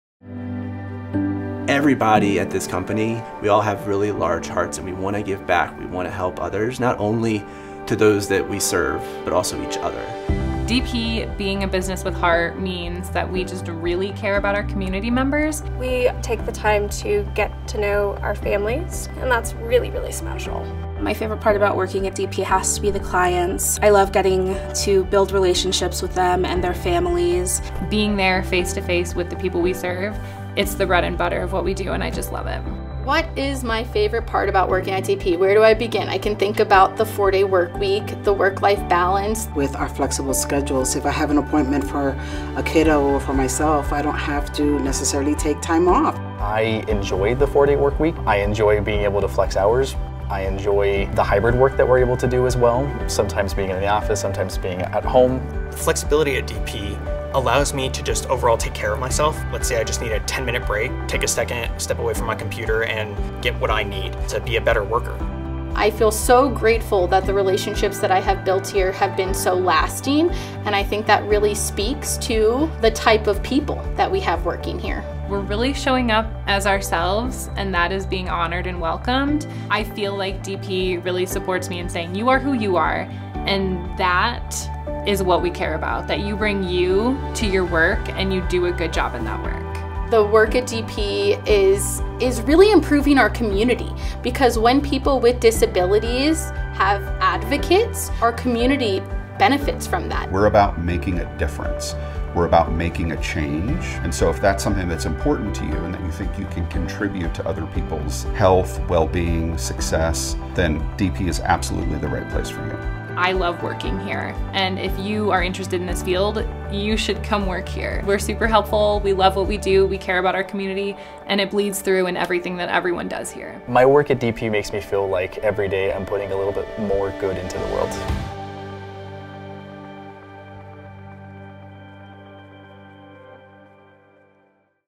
Business with Heart | Careers at DP
At Developmental Pathways (DP), we are a "business with heart" that serves our community with care and compassion, with our shared values and purpose at the forefront.

We believe that our people are our greatest asset and want to give them the ability to grow and do what they love. We are committed to creating an inclusive and dynamic work environment where employees can thrive and make a meaningful impact.

If you are looking for a challenging and rewarding career with an organization that values integrity, innovation, partnership, and stewardship, you've come to the right place! to learn more and see open positions on our team.

DP has been a vital community-based support for individuals with disabilities since 1964. Today, DP proudly serves over 15,000 members of all ages and their families in Arapahoe, Douglas, and Elbert Counties.